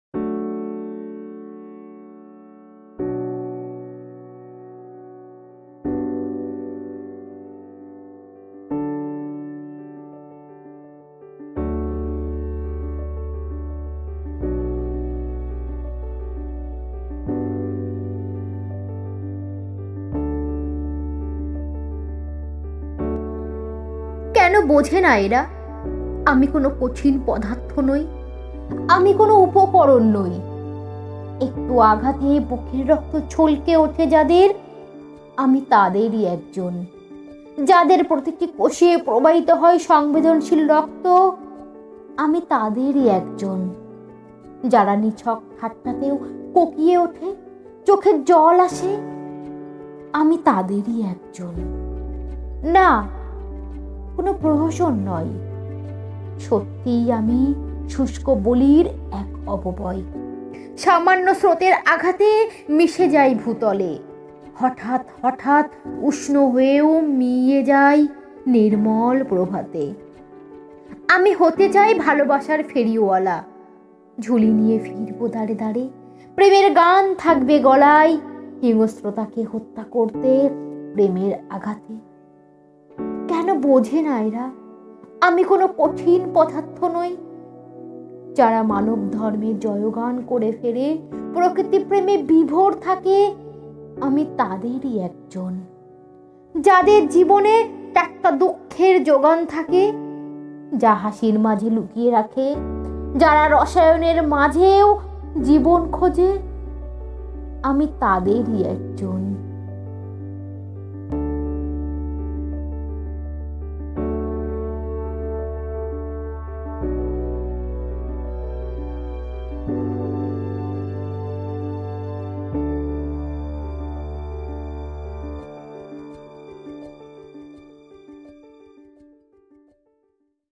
Ami Taderi Ekjon | Ratan Purkait | Recited by Soani | আমি তাদেরই একজন
বন্ধুরা...
আজ আপনাদের জন্য আমার কণ্ঠে, কবি রতন পুরকাইতের লেখা কবিতা "আমি তাদেরই একজন"।

পুরোটা শুনে দেখুন, নিশ্চয়ই ভালো লাগবে। ভিডিওটি প্রচুর প্রচুর শেয়ার করুন। লাইক করুন। আপনাদের মতামত জানান কমেন্টে।

পাশে থাকুন।
সাথে রাখুন।

ধন্যবাদ 🙏

বি: দ্র: - ছবিগুলো ইন্টারনেট থেকে সংগ্রহ করা।